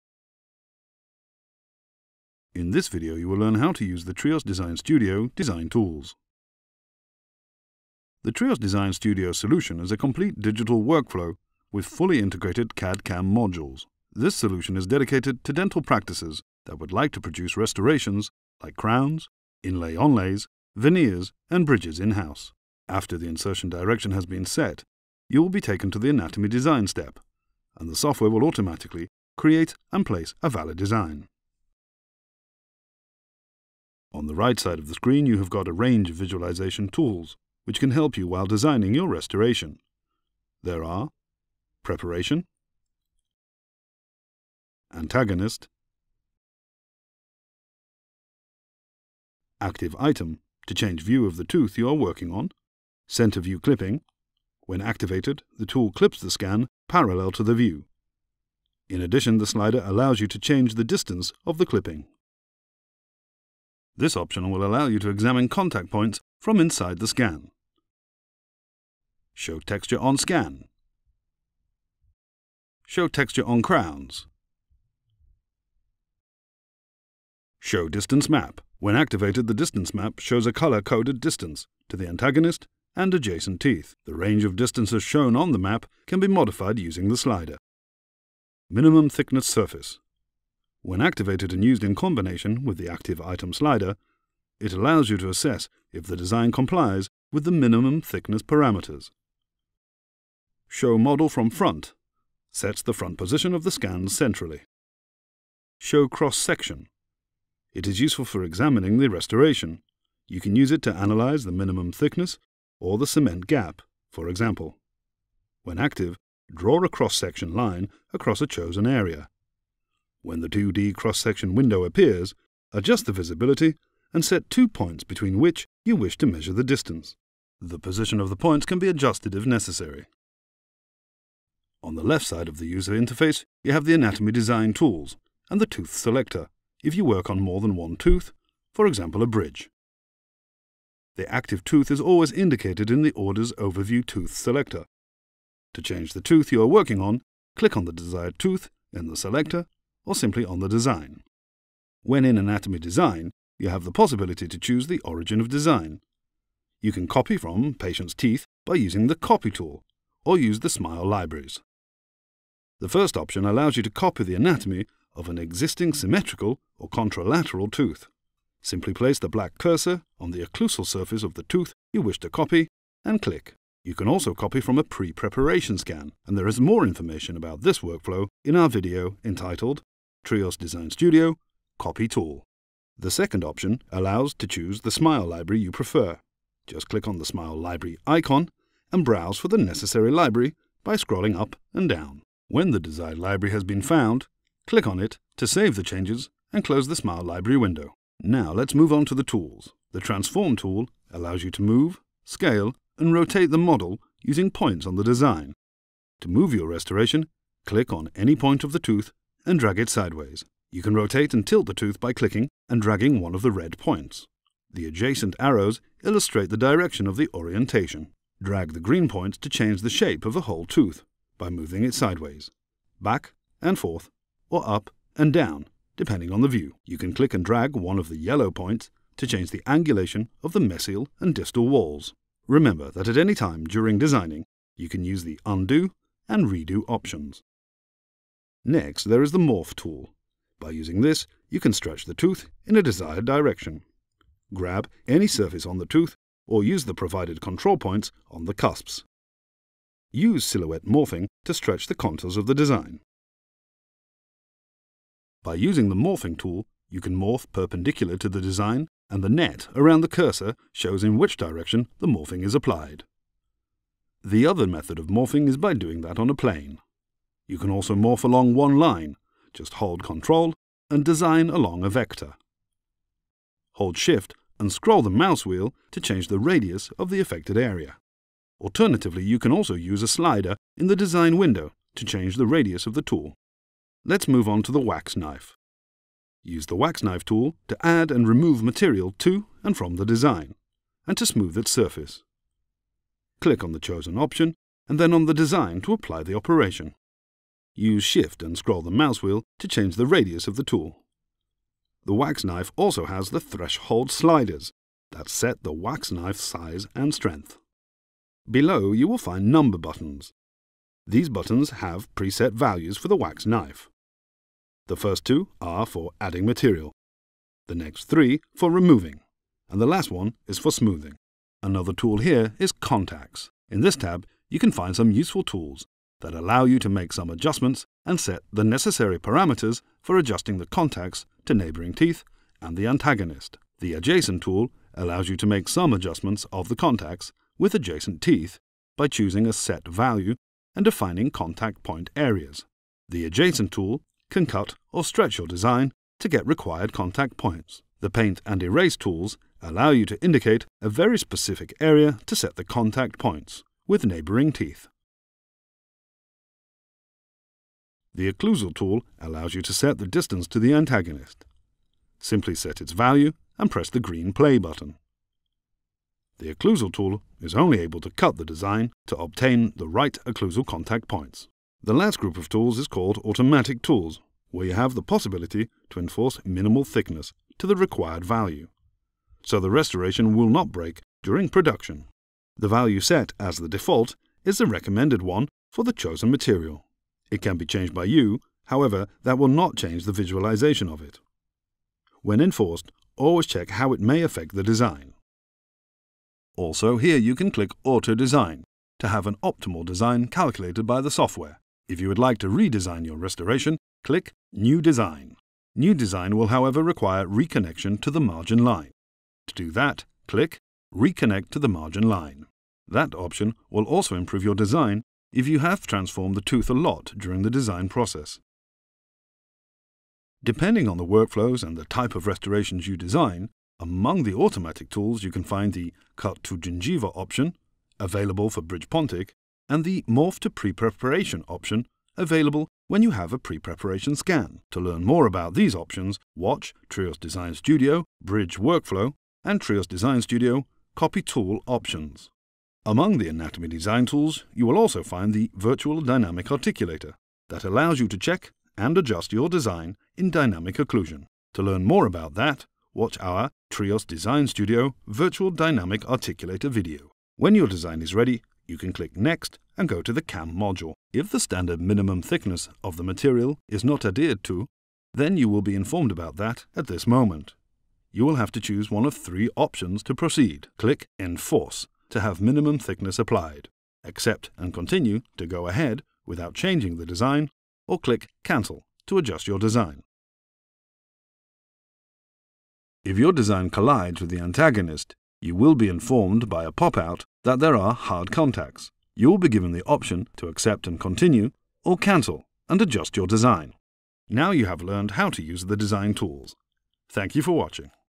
3Shape TRIOS Design Studio - Using Anatomy Design
In this video, you will learn how to use the TRIOS® Design Studio – Design Tools.